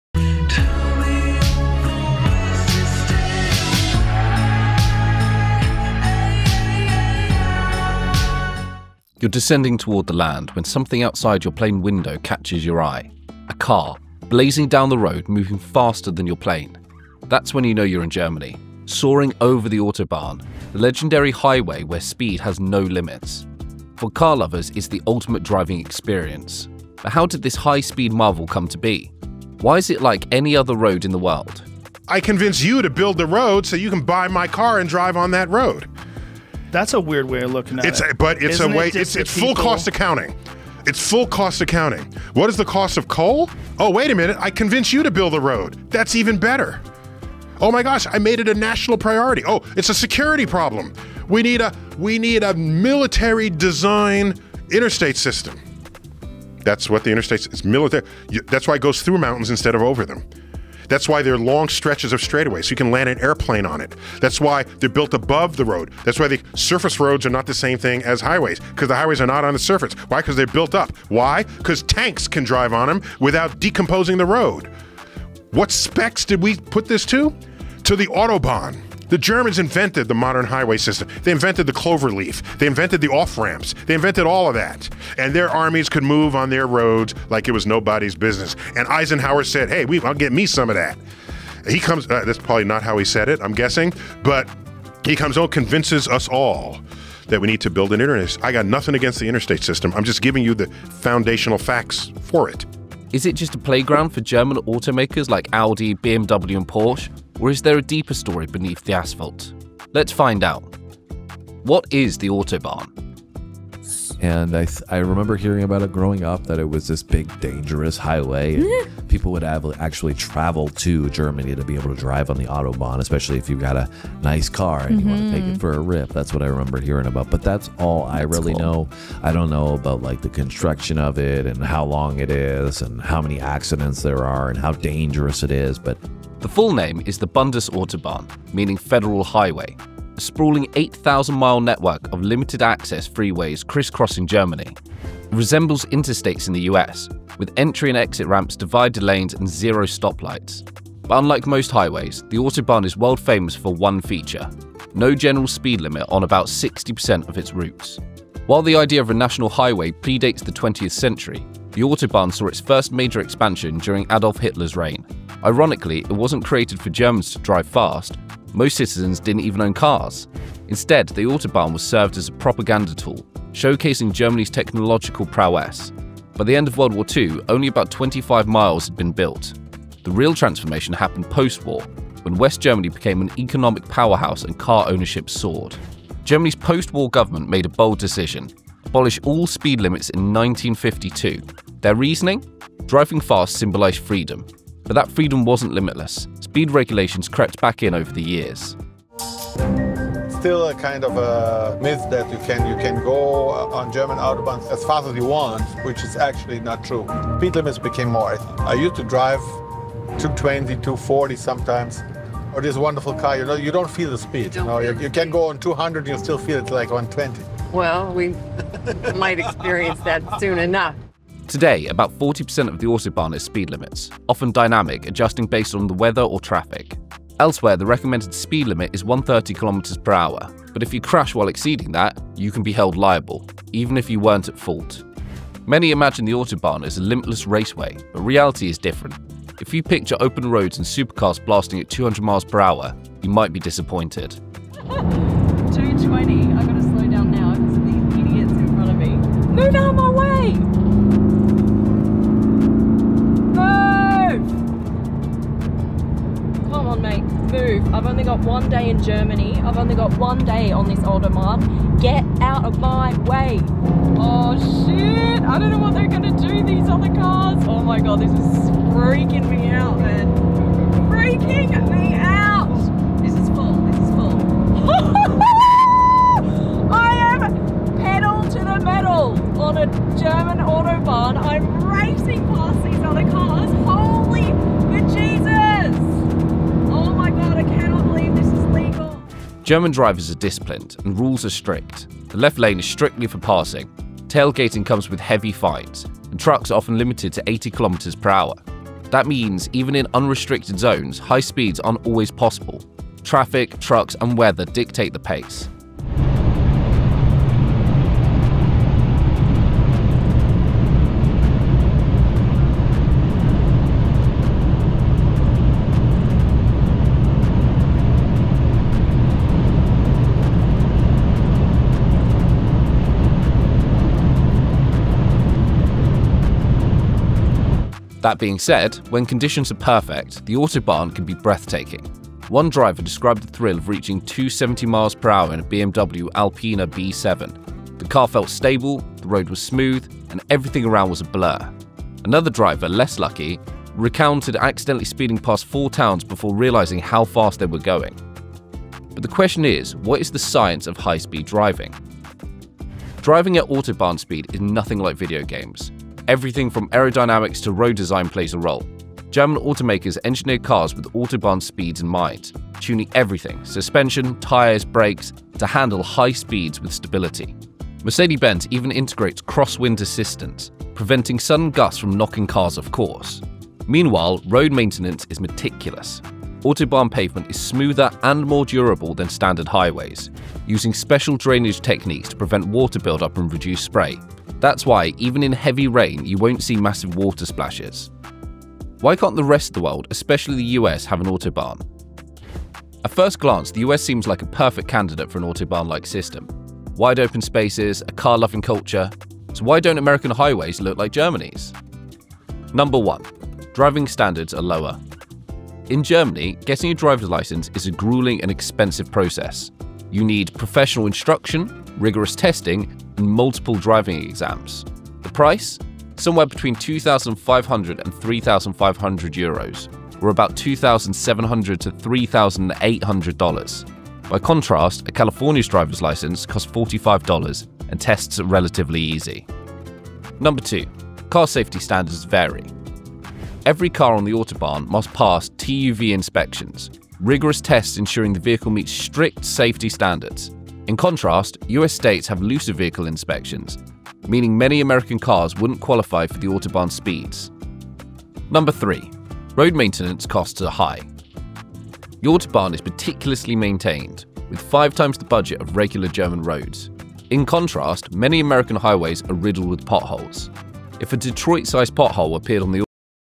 German Autobahn - Why is there no Speed Limit
🚀 German Autobahn - Why There Is No Speed Limit! 🏎️💨

Imagine looking out your plane window and seeing a car blazing down the road faster than you’re descending. That’s when you know—you’re flying over Germany’s Autobahn, the legendary highway where speed limits don’t exist on most stretches.

But why does Germany allow this? Is the Autobahn just a paradise for BMWs, Audis, and Porsches, or is there a deeper reason behind its no-speed-limit policy?

In this video, we break down:
✅ The History – How the Autobahn went from a political tool to a high-speed wonder
✅ No Speed Limit? – The real reason why 60% of it remains unrestricted
✅ The Science of Speed – How German engineering makes it possible
✅ Why the U.S. Can’t Have an Autobahn – The 4 major reasons
✅ Is It Actually Safe? – Autobahn vs. other highways

🚗 Would you drive on the Autobahn? Or should speed limits exist everywhere?

more deep dives from PriceyPursuits!